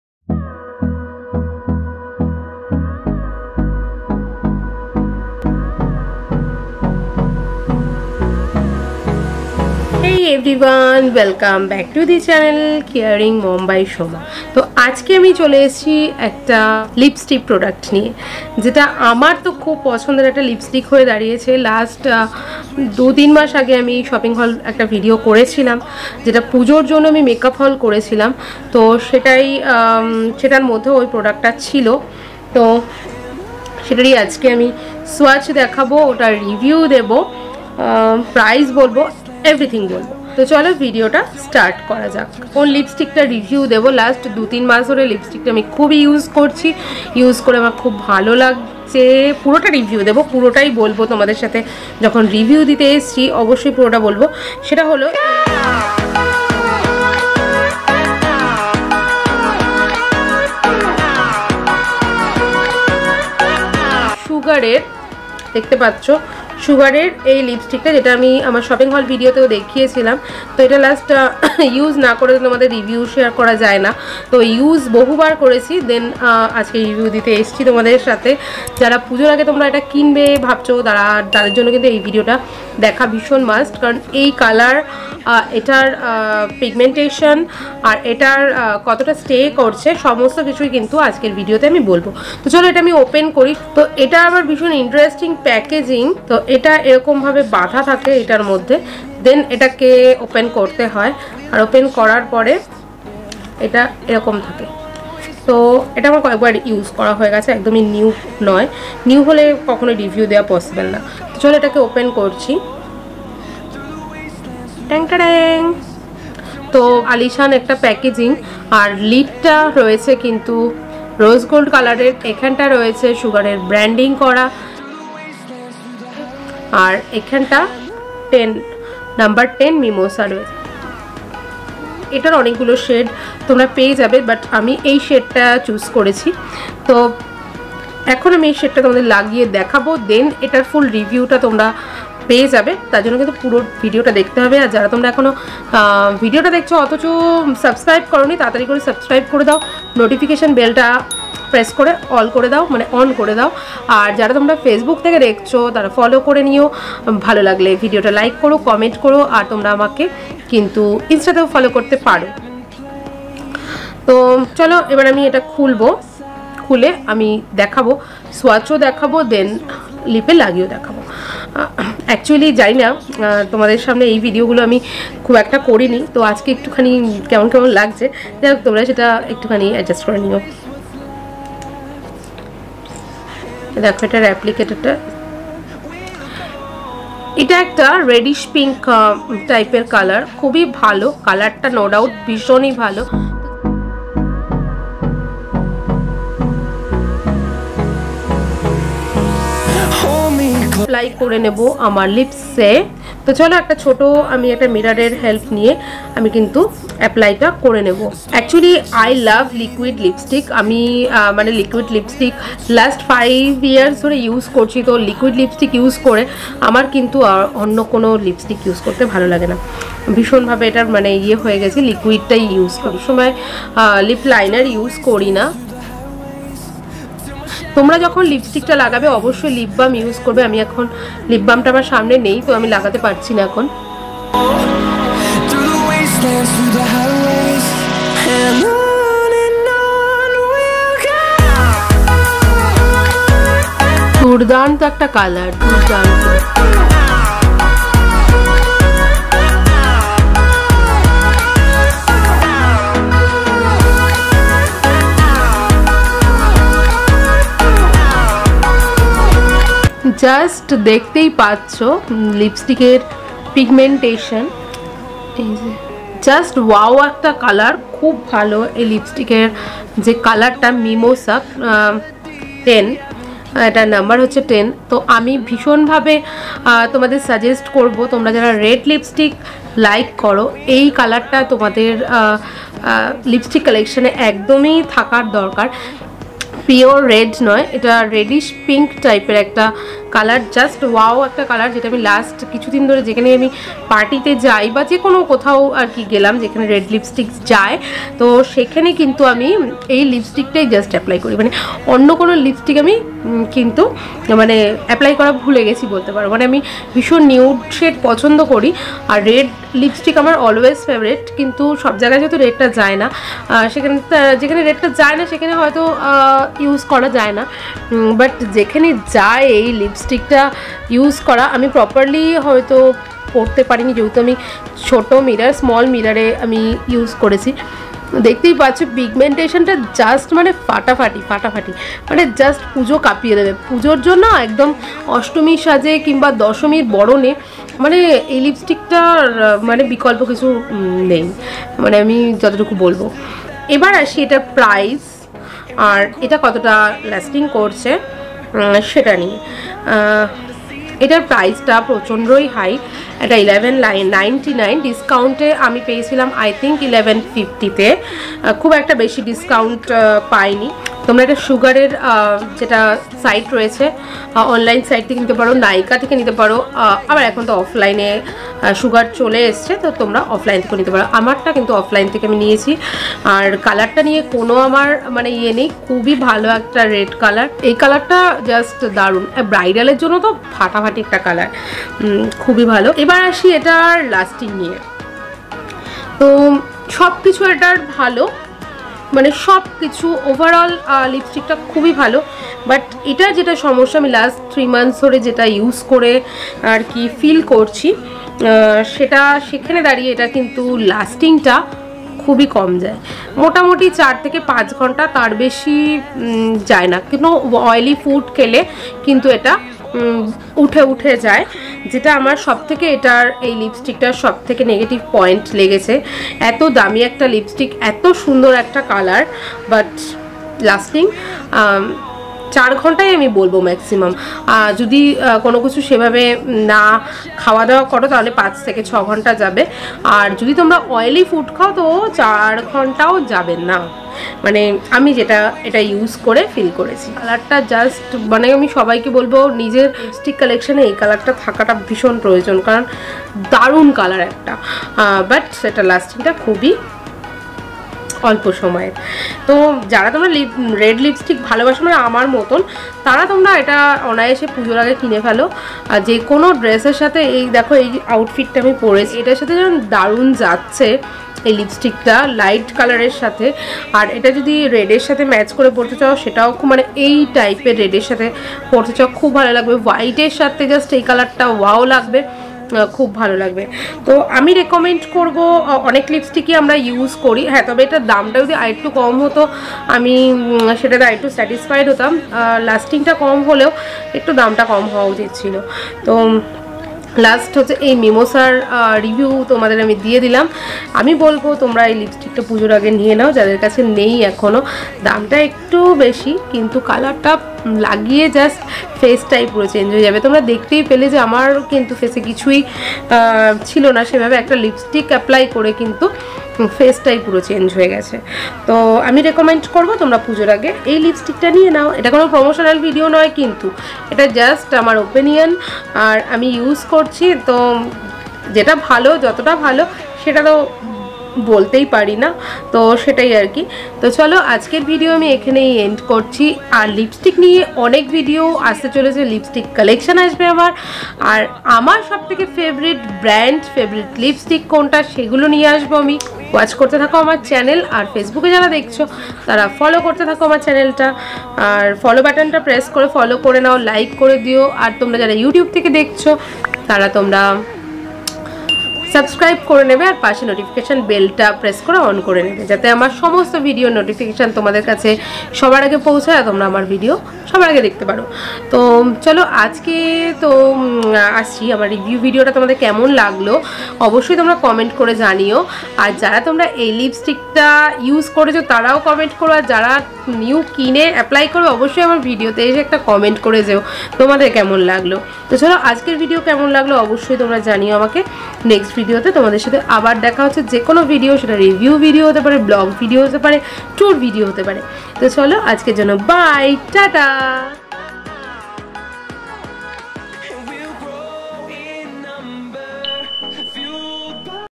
Best Red Lipstick | Sugar Mettle Liquid Lipstick Mimosa 10 Swatches & Review
Best Red Lipstick || Sugar Mettle Liquid Lipstick || Sugar Mettle Liquid Lipstick Mimosa 10 Swatches & Review || Sugar Mimosa 10